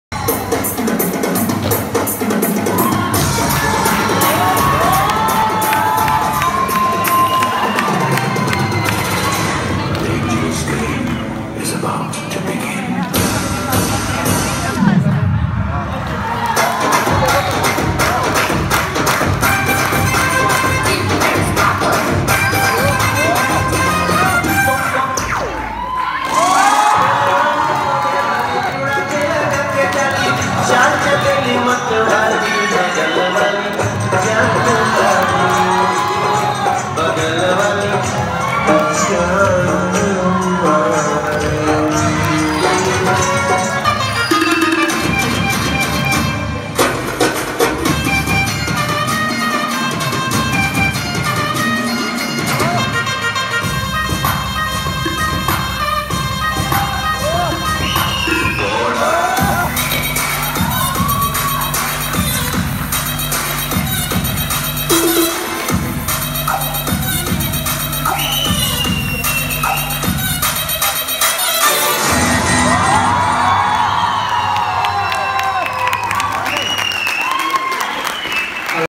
National Dance Sports Group Dance Champion 2017-18 ( Bengal Team )
Bengal Dance Sports Group Team win Gold Medal in National Dance Sports Championship 2017-18 at D.A.V. College , Chandigarh